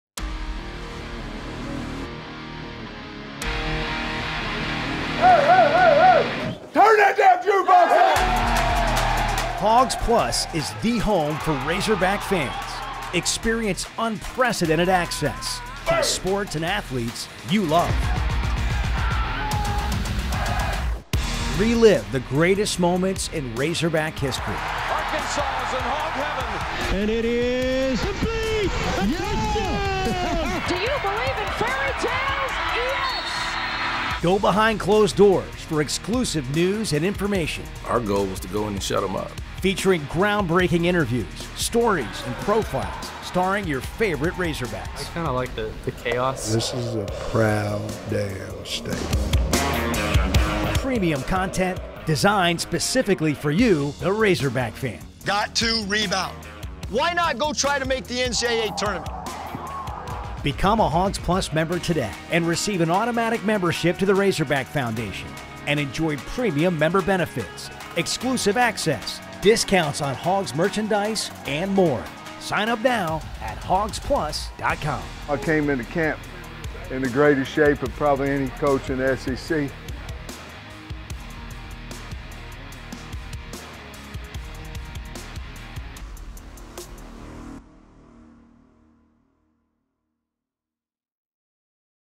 Welcome to Hogs+
Hogs+ is more than a subscription. It's an annual membership community for only $7.99 per month (+ fees & taxes) that offers Razorbacks fans access to exclusive premium content, complimentary Razorback level membership (a $50 value) to the Razorback Foundation, and many other additional member benefits.